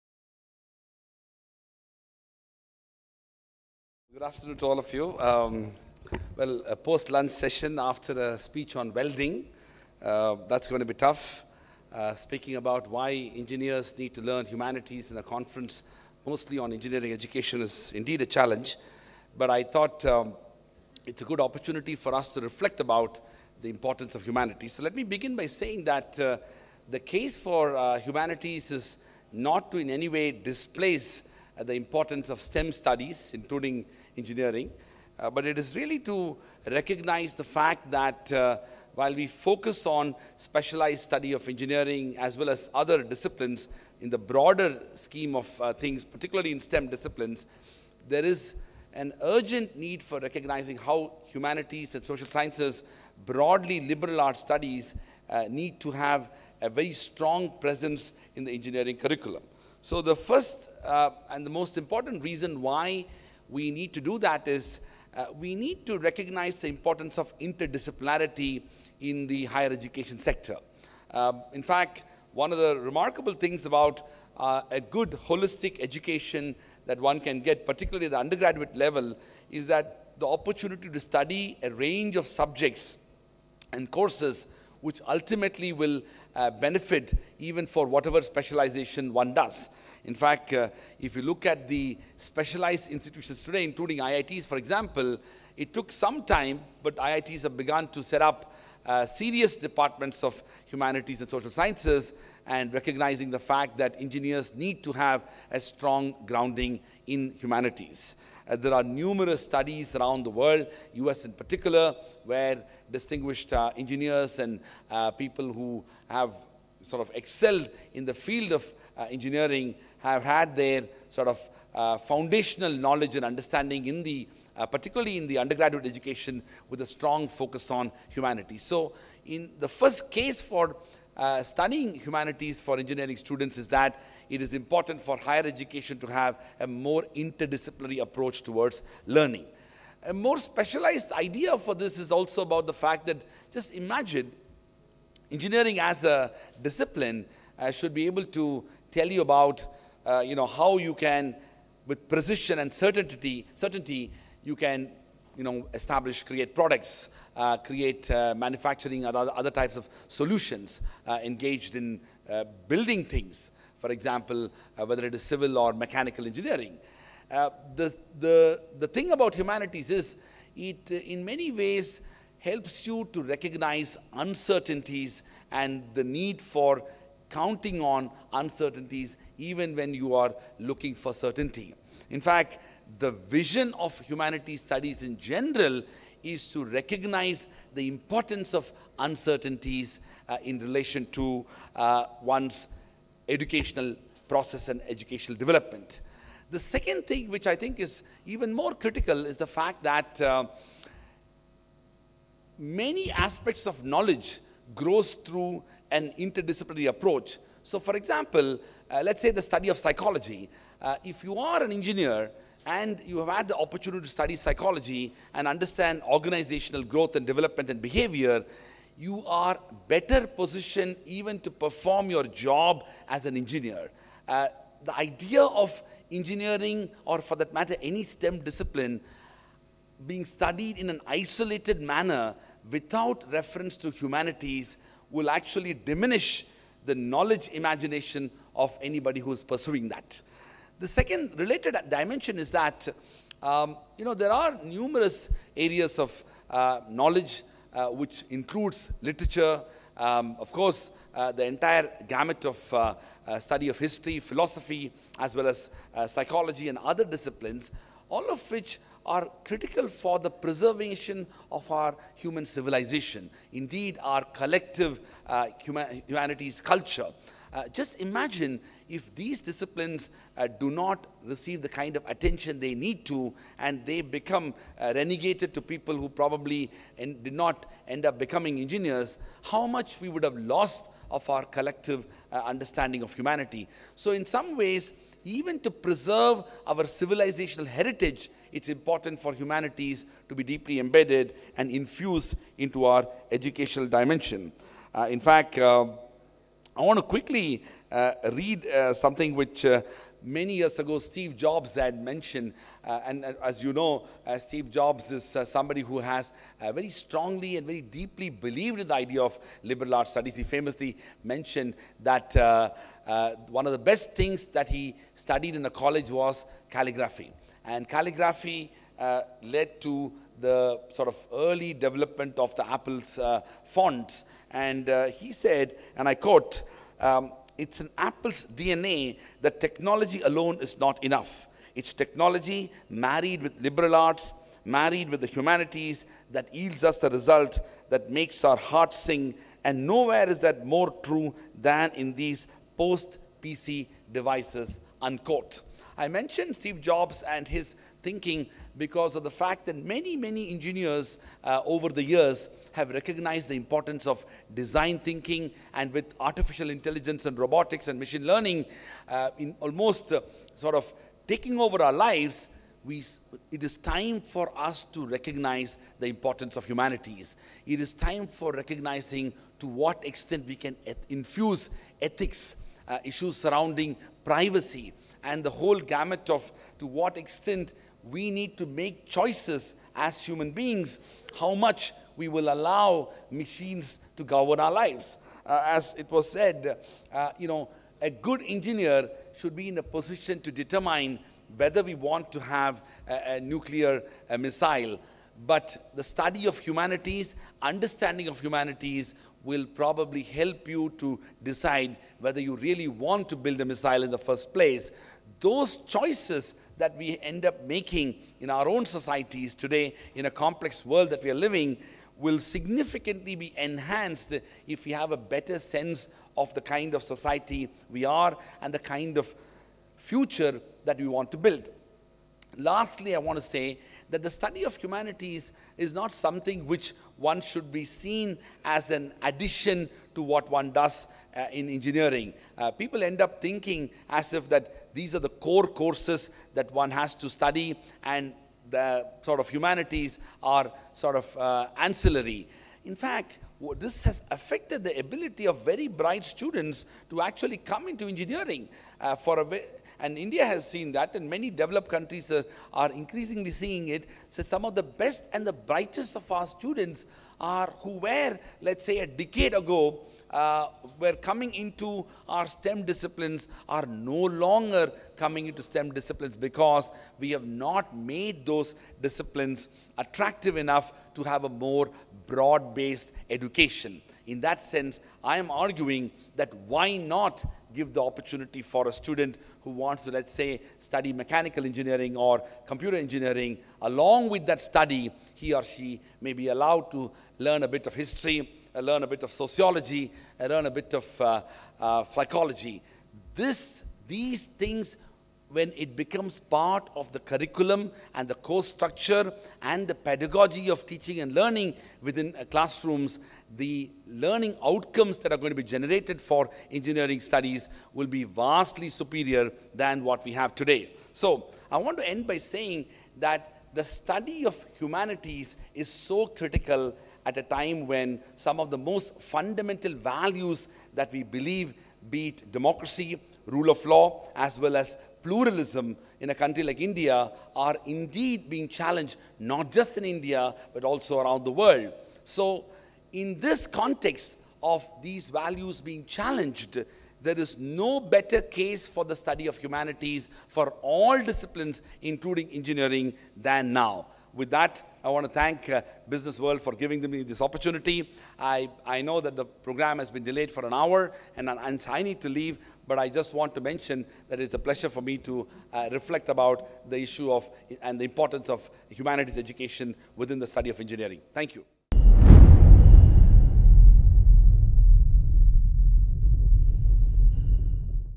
Is Humanities Necessary For Engineering?
A session held at BW Businessworld Engineering Excellence Conclave unveiled the importance of humanities in the course of Engineering by Dr. C Rajkumar, Vice Chancellor of O.P. Jindal University, who emphasized on the topic “Why engineers should have strong foundational knowledge in humanities”. He also talked about numerous studies held all around the world that explains the inter-relationship between Engineering and Humanities which helps build a favorable and bright future for the upcoming engineers